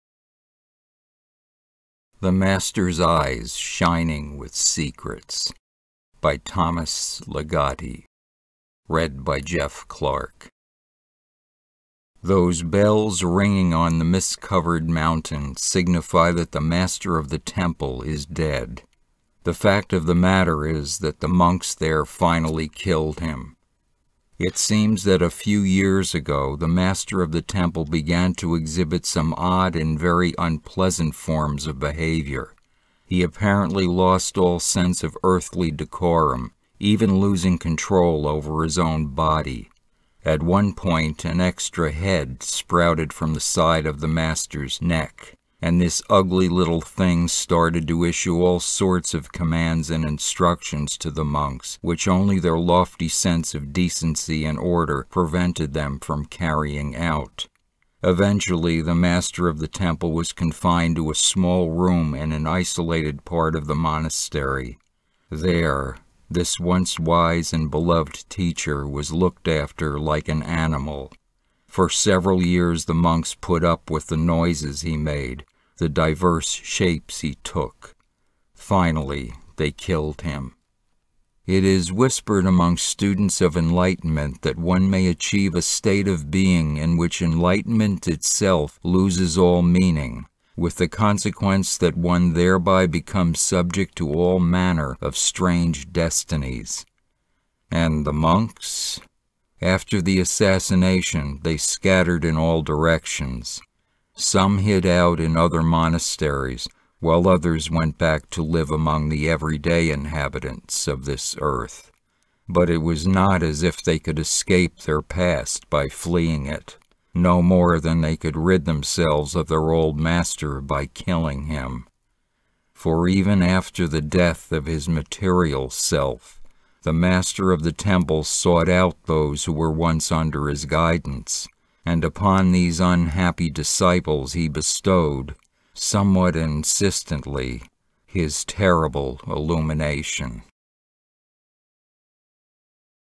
Notebook of the Night 1: THE MASTER'S EYES SHINING WITH SECRETS by Thomas Ligotti
First in the third section of short pieces in the collection NOCTUARY.

The photo is of an Italian monastery in Piemonte by Martha Bakerjian, to accompany her article